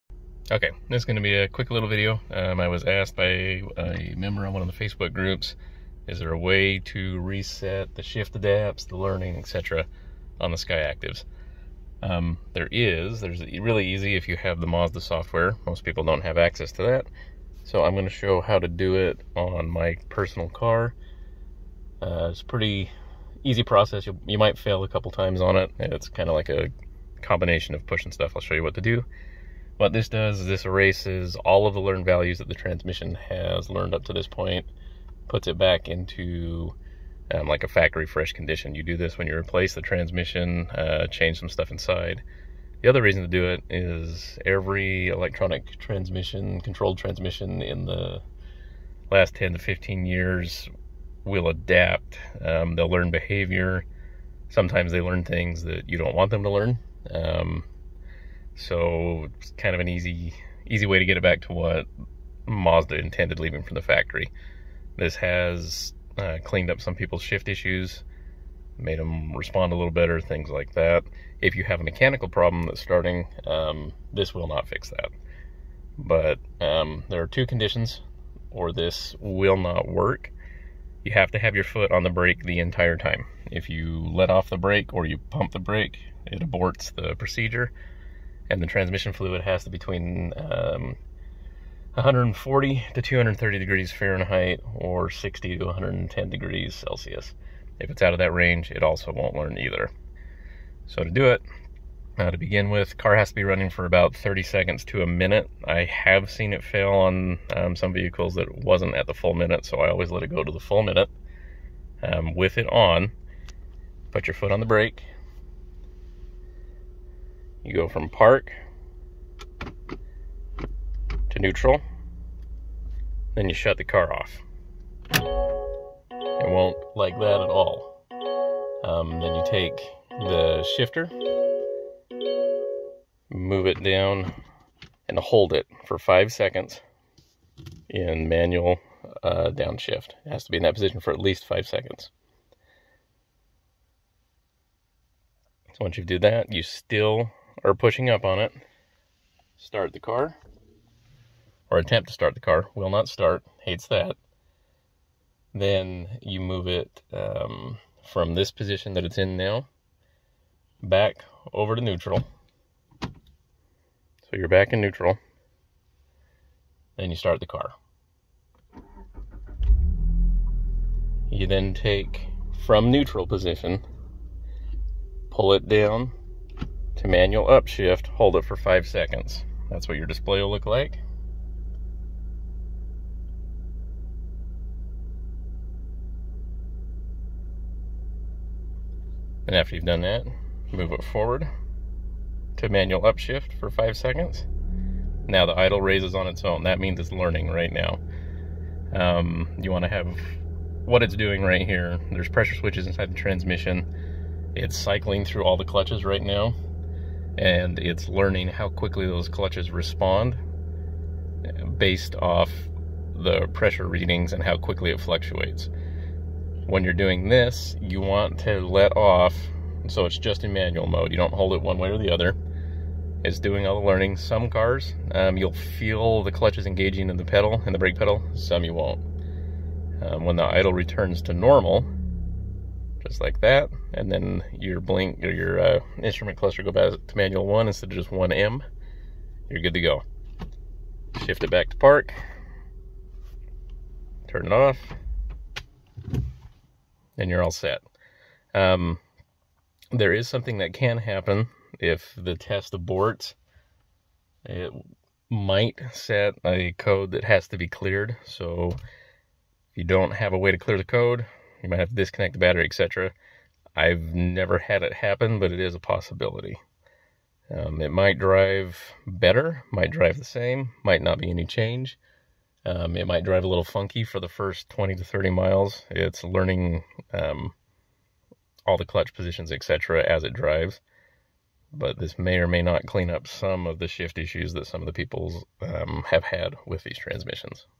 Mazda Skyactiv AT initial learning mode.
This is a quick video to show how to put a Mazda Skyactiv transmission into initialization mode. This clears all the shift adapts and learned clutch values. Unhooking the negative battery cable will not do this! This needs to be done if you have replaced the transmission or internal components. It is also useful to relearn the transmission if it has picked up "bad" behaviors.

Perform the following steps with the brake pedal depressed:

*you need to ugga dugga the brake pedal, not enough pressure and it will NOT work*

a. Shift the selector from the P to N position, press the push-button start one time and stop the engine.

b. Shift the selector from the N position to M(-) position. Press the push button start one time while holding the lever in the M(-) position. Wait for 5 seconds or more (ignition switch ON /engine off.

C. Shift the selector from M(-) to N position. Press the push-button start one time to start the engine.

d. Shift the selector lever from N position to M(+) position and wait for 5 seconds or more while holding the lever in the M(+) position.

e. Shift the selector lever from M(+) position to M(-) position and wait for 5 seconds or more while holding the lever in the M(-) position.

f. When idle increase starts, shift the selector lever to the M position (manual position), and verify that M1 is displayed after the shift position indicator light flashes two times. Watch carefully because the light flashes only one time.